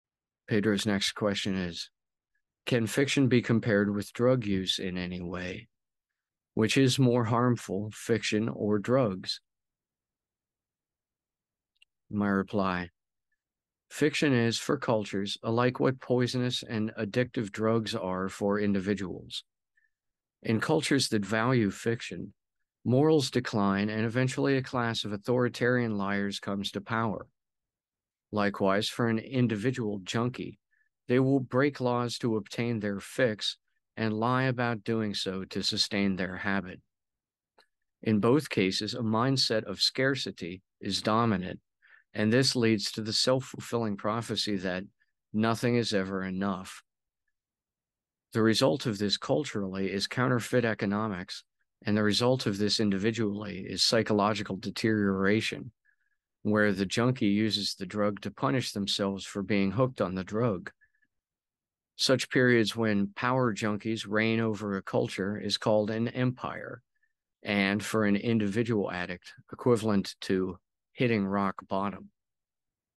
Which Is Worse - Fiction Or Drugs?
POD cast 004 clip 7

Can fiction be compared with drug use in any way? Which is more harmful, fiction or drugs?

Fiction is, for cultures, alike what poisonous and addictive “drugs” are for individuals. In cultures that value fiction, morals decline and eventually a class of authoritarian liars comes to power; likewise, for an individual “junky” they will break laws to obtain their “fix” and lie about doing so to sustain their “habit.” In both cases, a mindset of scarcity is dominant and this leads to the self-fulfilling prophecy that “nothing is ever enough.” The result of this culturally is counterfeit economics and the result of this individually is psychological deterioration, where the “junky” uses the drug to punish themselves for being hooked on the drug. Such periods when “power junkies” reign over a culture is called an “empire” and, for an individual addict, equivalent to “hitting rock bottom.”